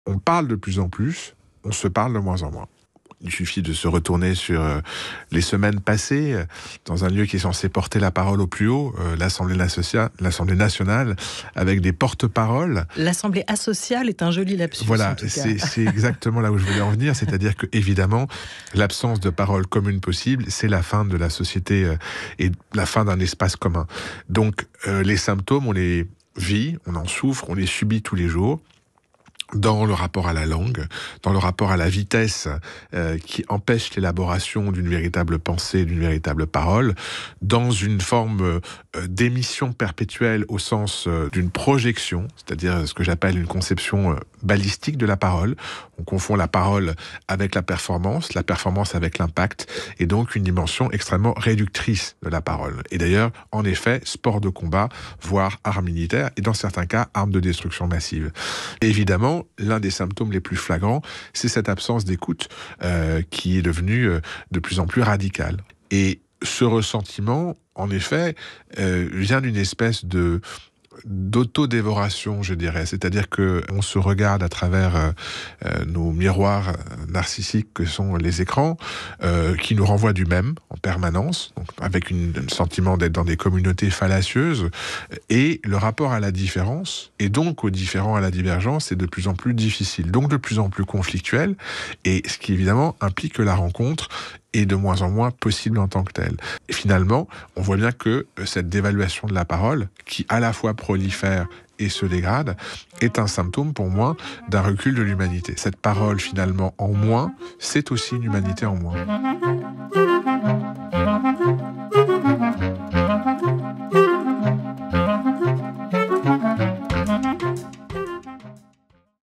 "On se parle de plus en plus, on s'écoute de moins en moins" : Gérald Garutti, metteur en scène
À l'ère du clash permanent et de l'impossibilité de débattre, le philosophe et metteur en scène Gérald Garutti rappelle que le dialogue, c'est ce qui fait notre humanité.